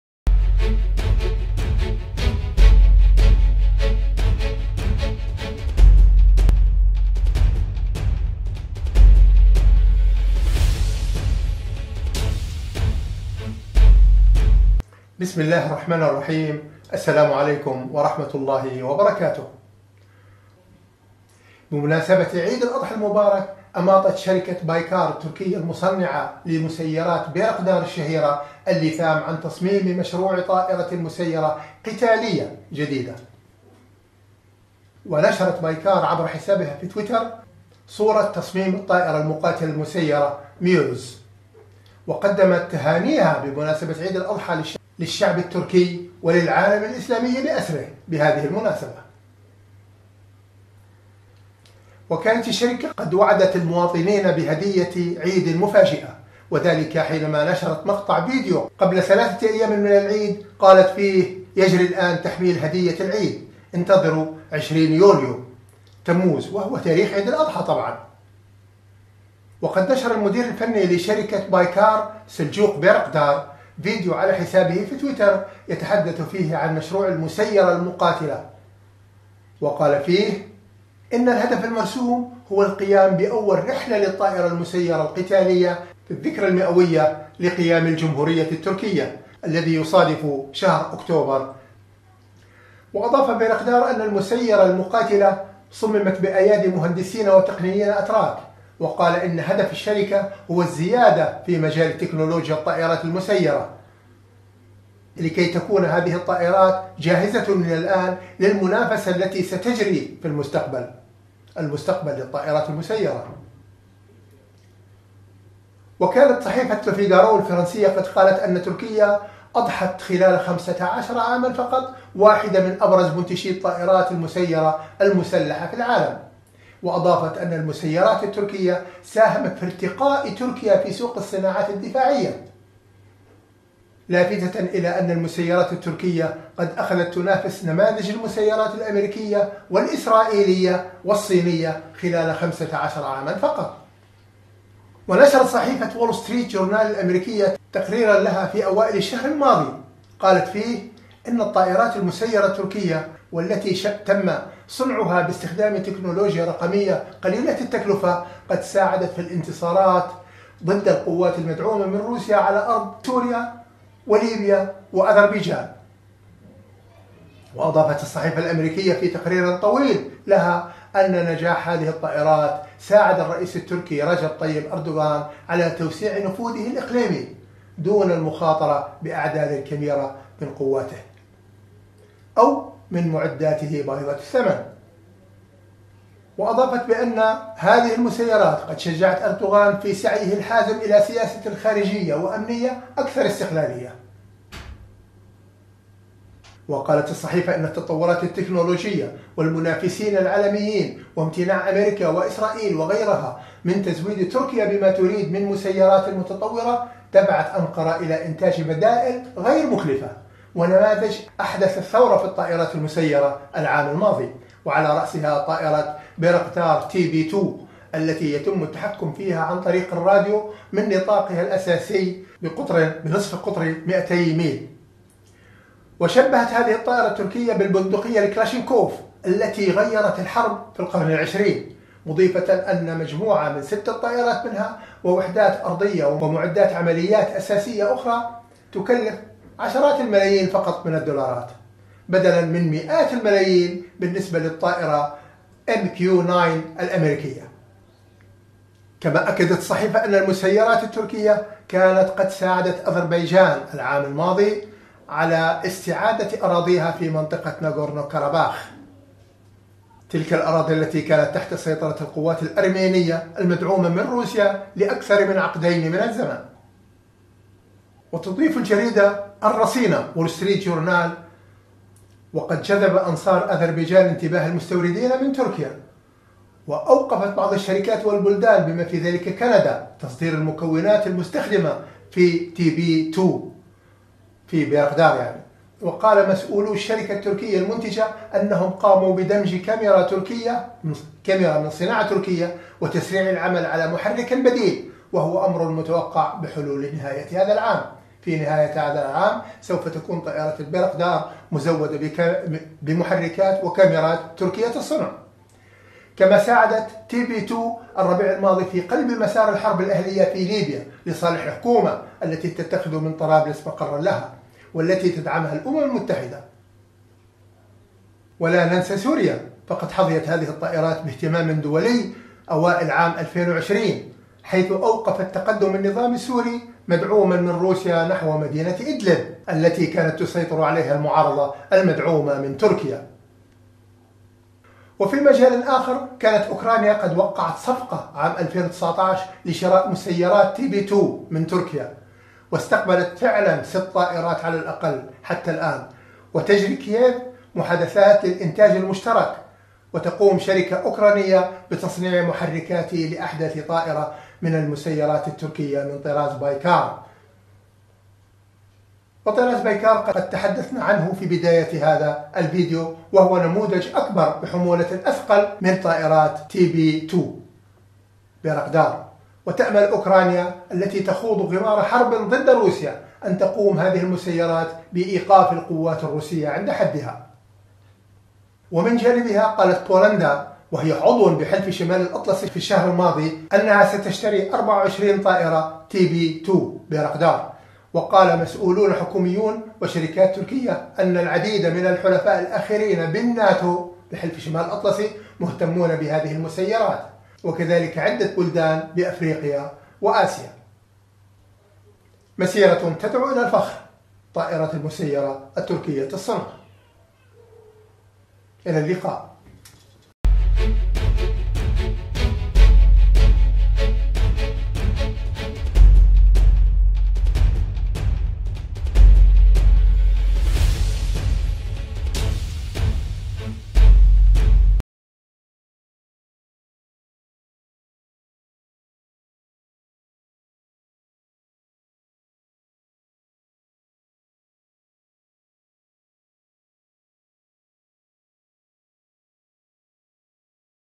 ميوز " طائرة مسيرة جديدة توقع الرعب في قلوب اعداء تركيا
بمناسبة عيد الأضحى المبارك، أماطت شركة "بايكار" التركية، المصنعة لمسيرات "بيرقدار" الشهيرة، اللثام عن تصميم مشروع طائرة مسيرة قتالية جديدة.
ونشرت "بايكار" عبر حسابها في تويتر لأول مرة صورا لتصميم الطائرة المقاتلة المسيرة التي اطلقت عليها اسم  "ميوز"، وقدمت الشركة تهانيها بمناسبة عيد الأضحى للشعب التركي والعالم الإسلامي بأسره.
وكانت الشركة قد وعدت المواطنين بهدية عيد مفاجأة، وذلك حينما نشرت مقطع فيديو قبل ثلاثة ايام من العيد، قالت فيه:  "يجري الان  تحميل هدية العيد.. انتظروا 20 يوليو/تموز، وهو تاريخ عيد الاضحى المبارك، وبالفعل فقد تحقق ذلك واطلع الناس على صور هذه المسيرة القتالية الجديدة.